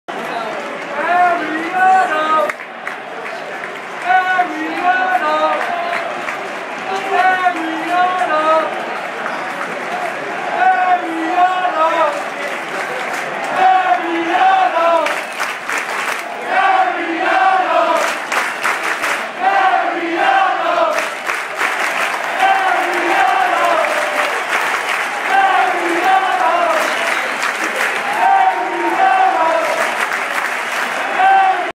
Mariano's last game at the Yankee's Stadium
Last game of Mariano with 50 000 fans scanding his name